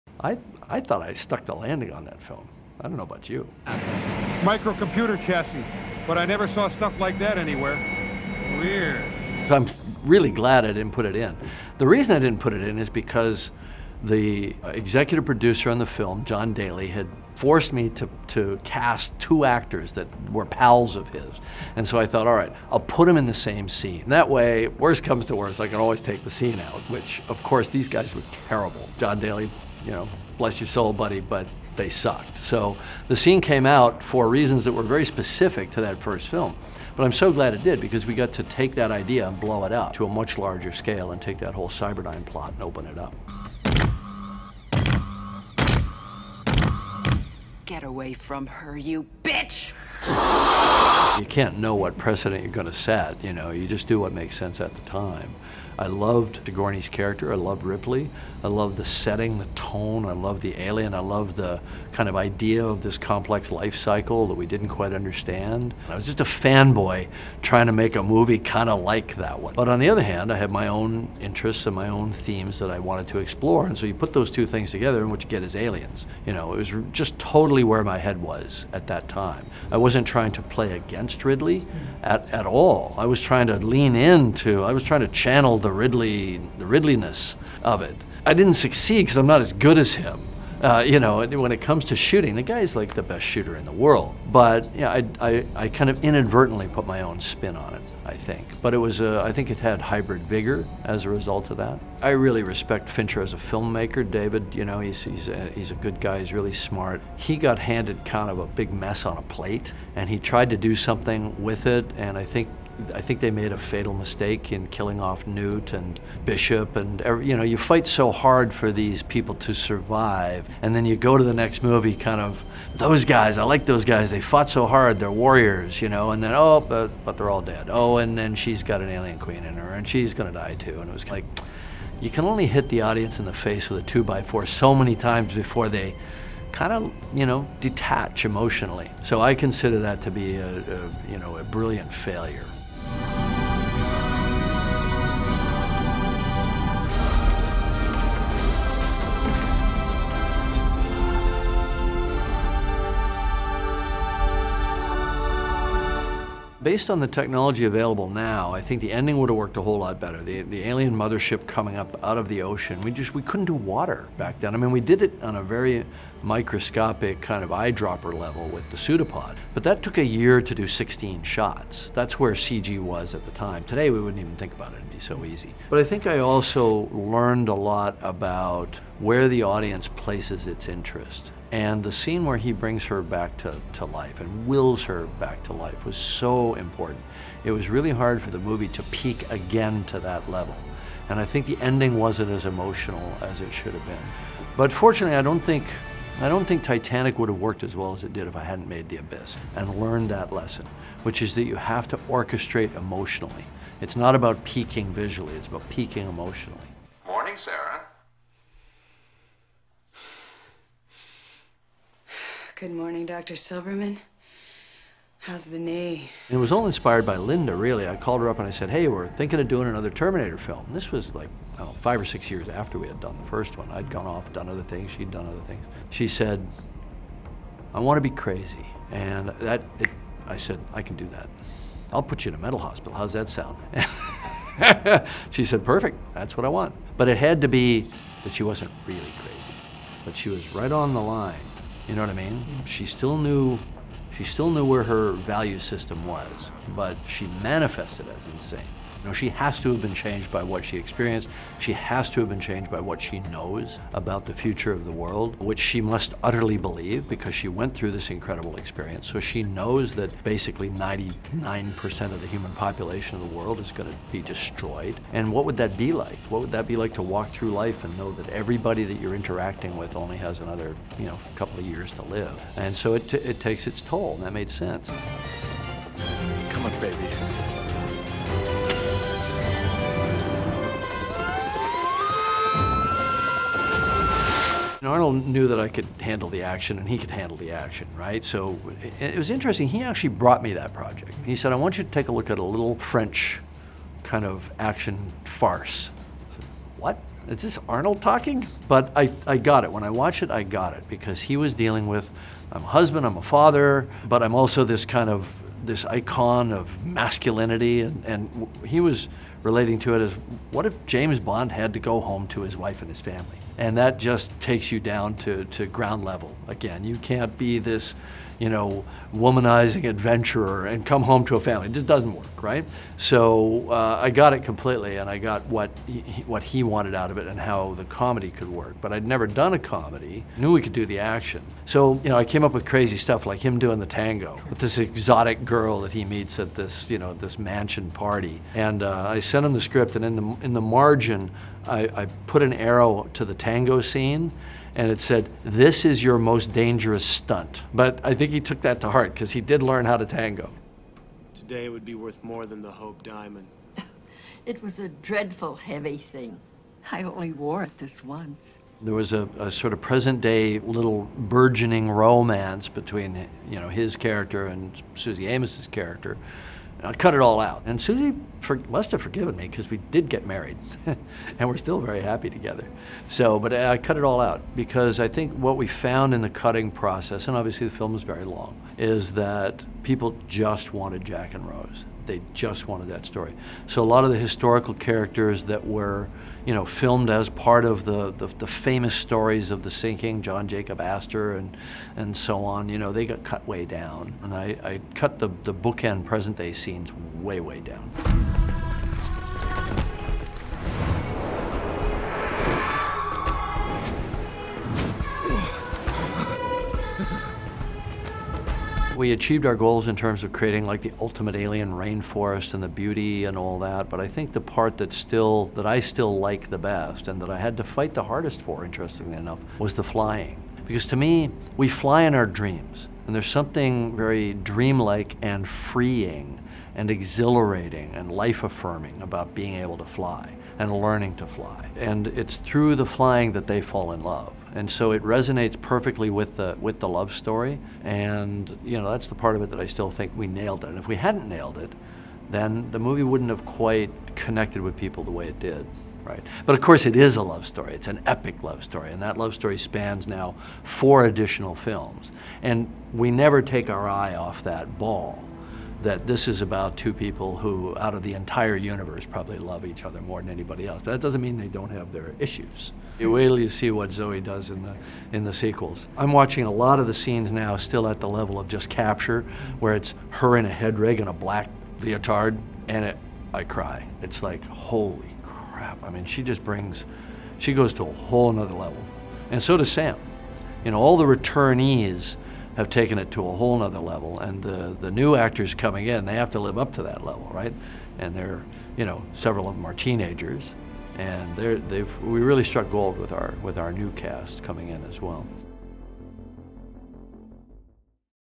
James Cameron on memorable scenes in 'The Terminator,' 'Titanic,' 'Aliens,' 'Avatar' and more
Director James Cameron talks about his famous films including 'The Terminator,' 'Alien' series, and 'Titanic.' Cameron talks about scenes that were changed or cut, and what's next for 'Avatar.'